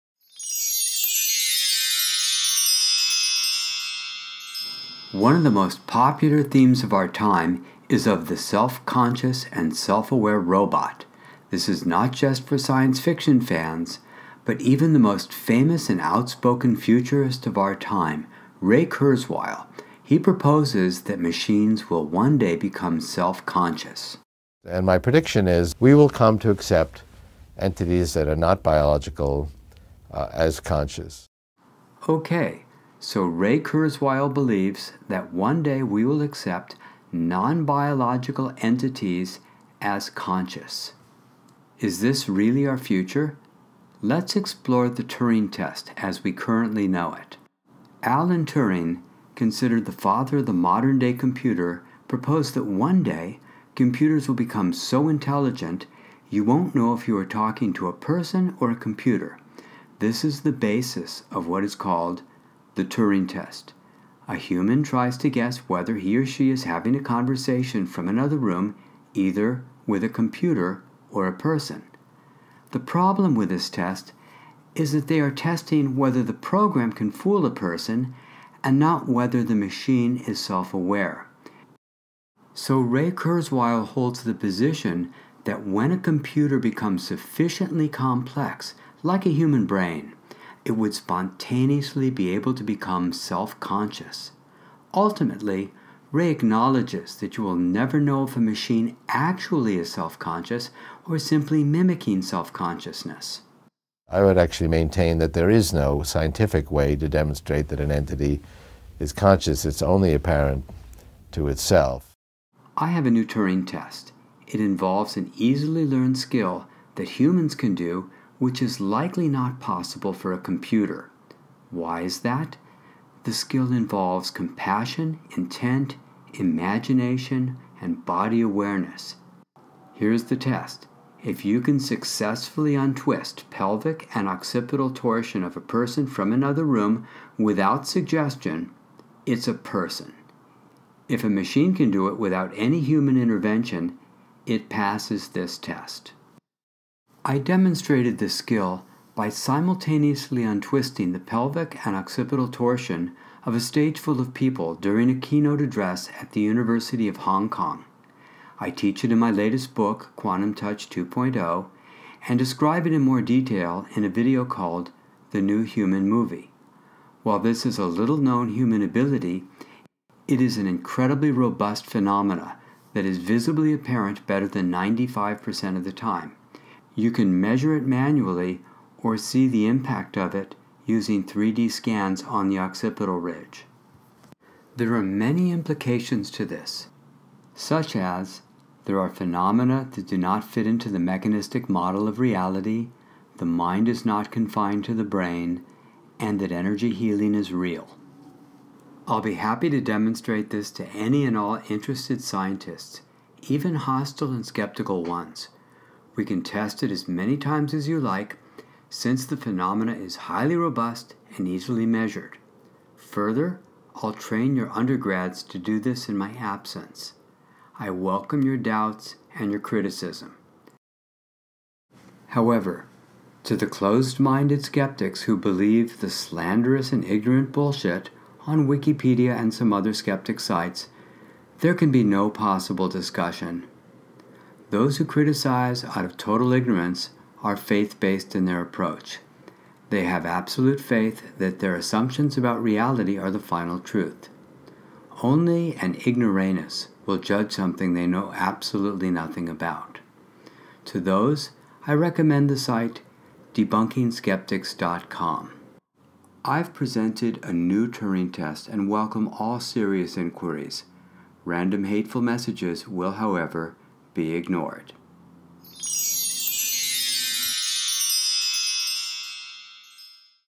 Proposing A New Turing Test
The Turing Test only informs us that a computer program was  able to fool a person into thinking that he or she had communicated  to a real person.  It does not test for self-consciousness.  I propose a new Turing Test that can make that determination.